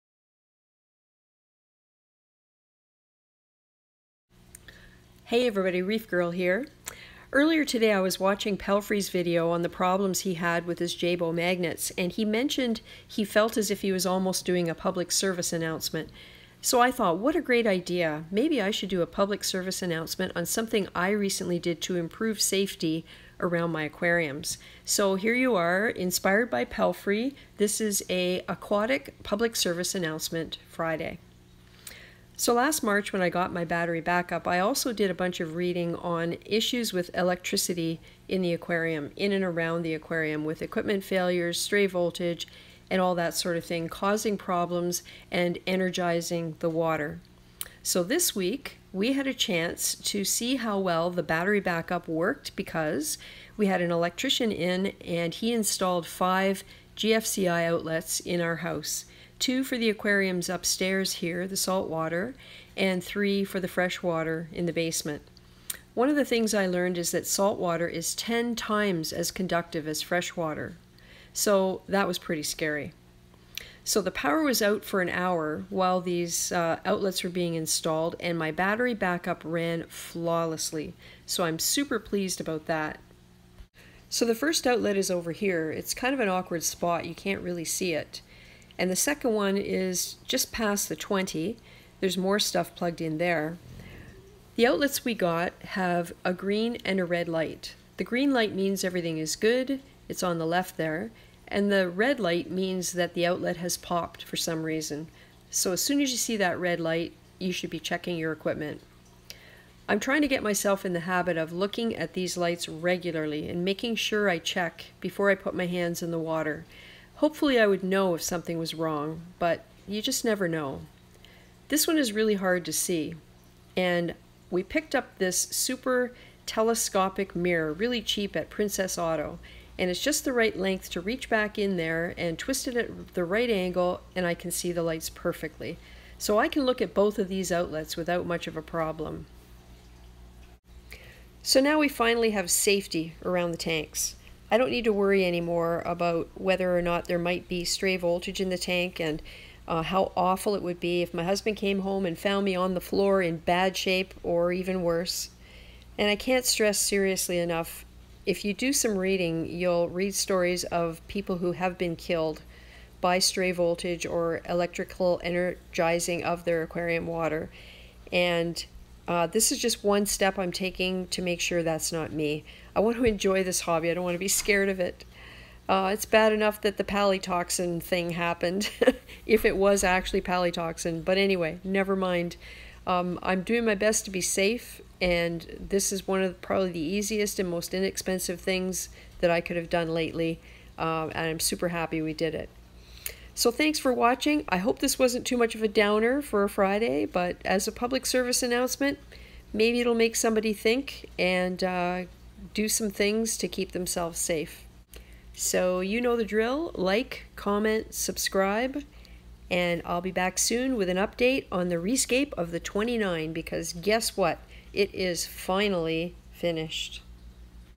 Aquatic PSA Friday - GFCI Outlets for Aquariums
Inspired by Pelphrey's comment that he felt he was making kind of a public service announcement in his video about the jebao magnet problem. I thought, what a good idea - so I thought I'd do a PSA about safety. I hope you find this useful and maybe even a bit thought-provoking.